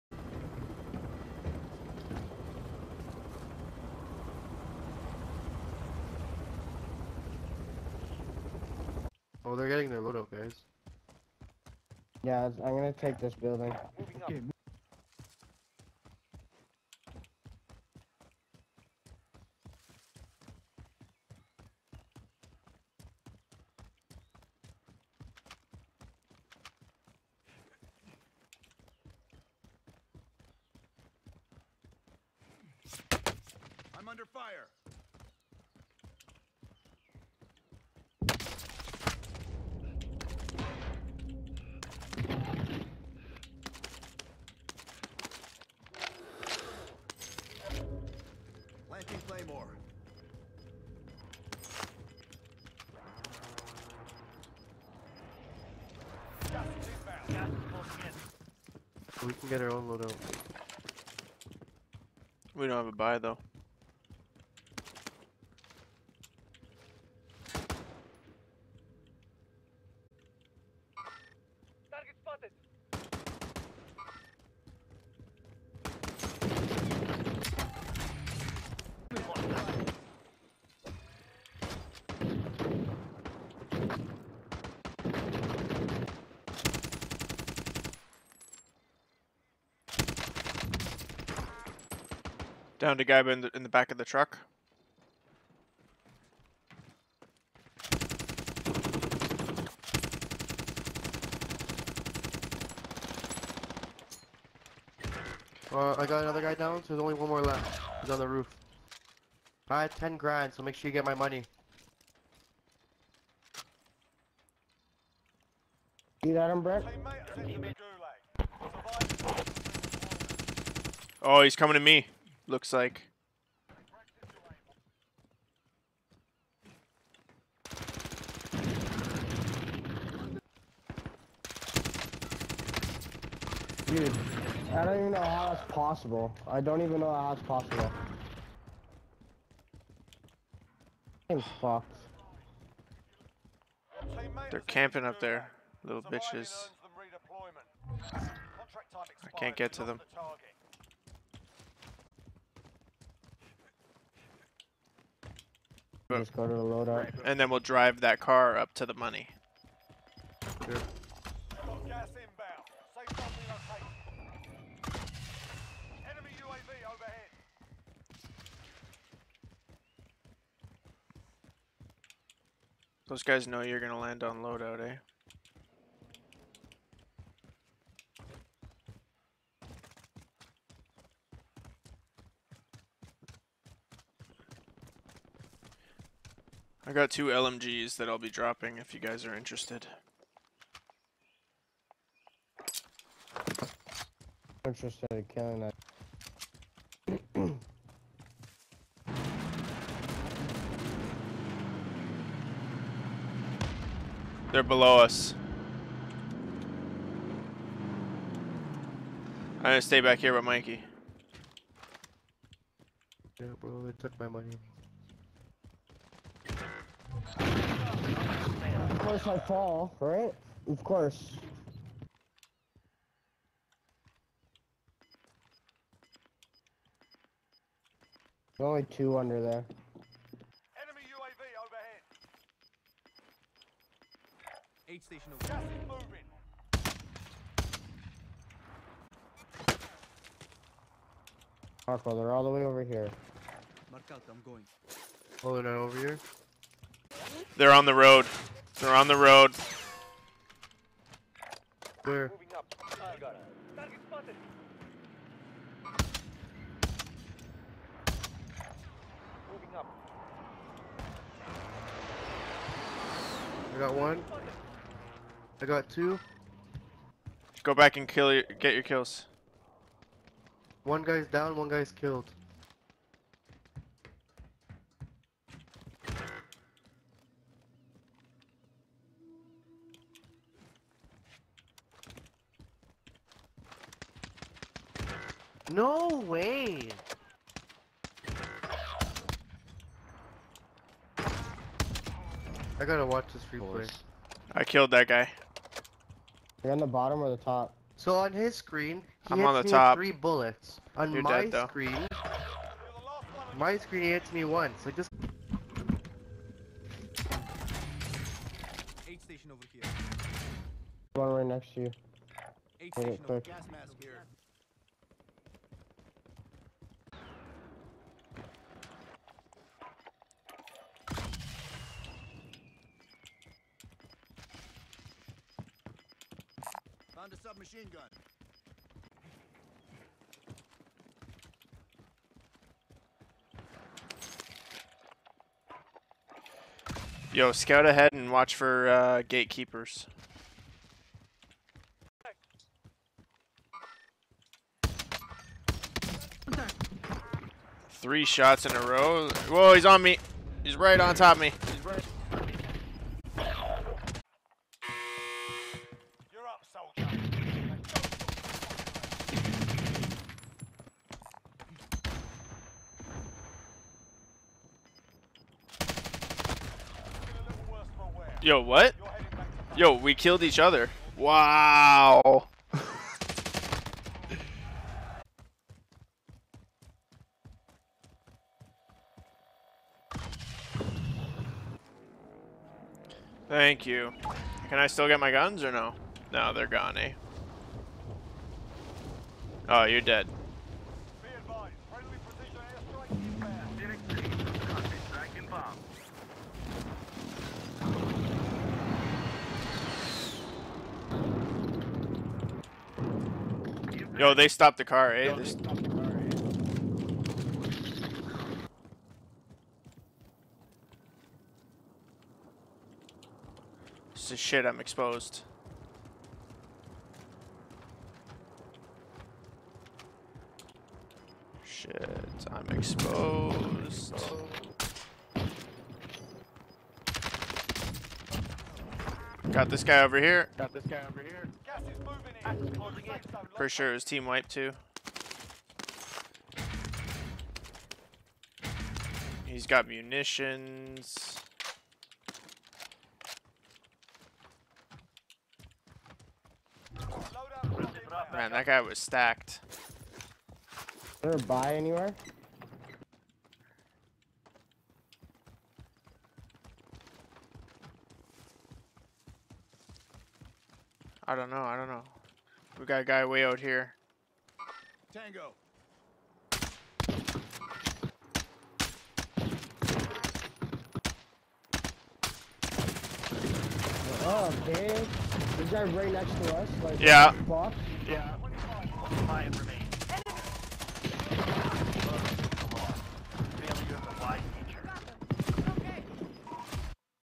Cof: TheBetterBlowerUpper Baby! Highway Murderers and No-Damage RPGs
Consistent Hellfire on the Overpass

Gulag Life Trading

RPG&#39;s that do no damage?!?

Should have got at least One.